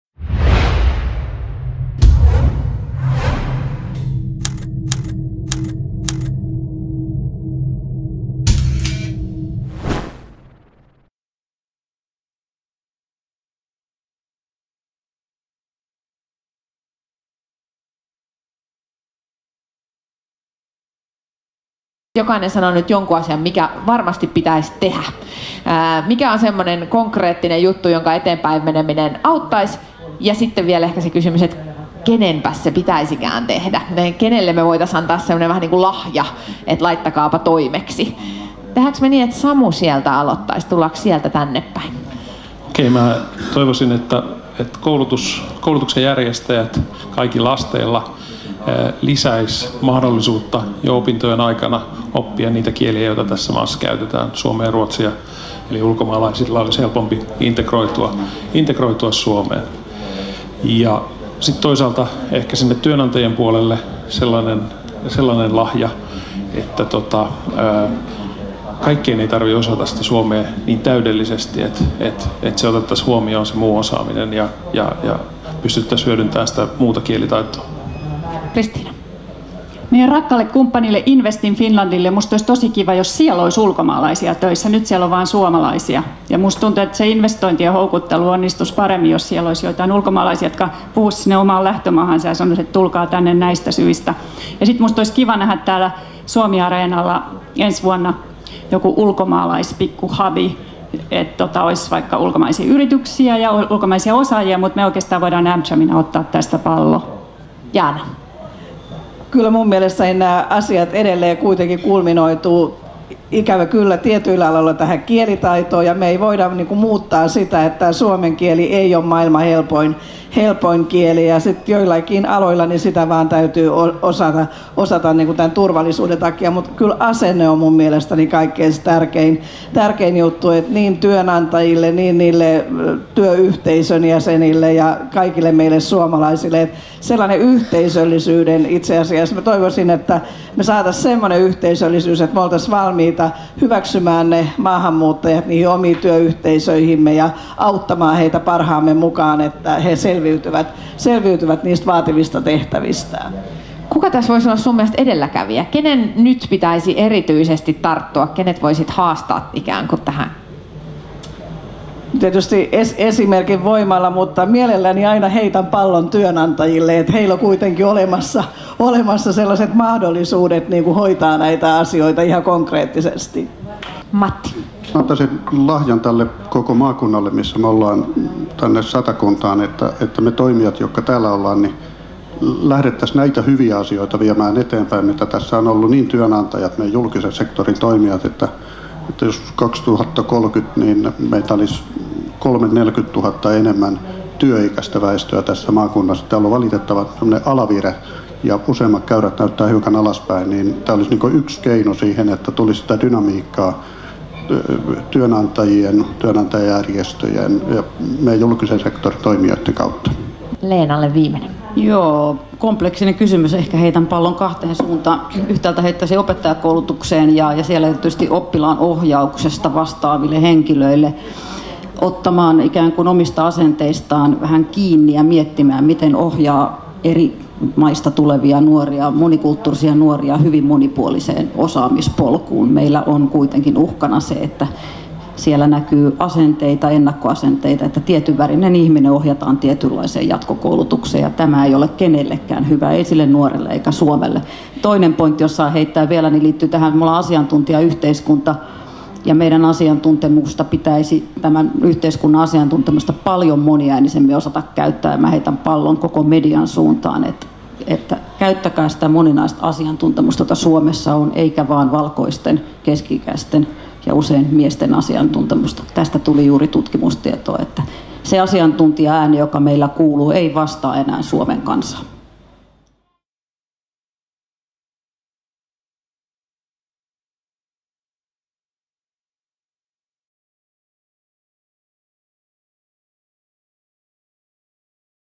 CIMO SuomiAreenalla 2015: Panelistien loppukommentit
Sanoista tekoihin. CIMOn "Osaako Suomi hyödyntää kansainvälistä osaamista" -paneelin loppukeskustelu SuomiAreenalla Porissa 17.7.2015. Juontaja: Tuuli Kaskinen, toiminnanjohtaja, Demos Helsinki; Kristiina Helenius, CEO, Amcham Finland; Matti Isokallio, kuntayhtymän johtaja, Sataedu; Jaana Laitinen-Pesola, kansanedustaja: Samu Seitsalo, johtaja, CIMO; Leena Suurpää, tutkimusjohtaja, Nuorisotutkimusverkosto.

Kuvaus ja leikkaus: Meri Lukkarinen
Copyright: CIMO 2015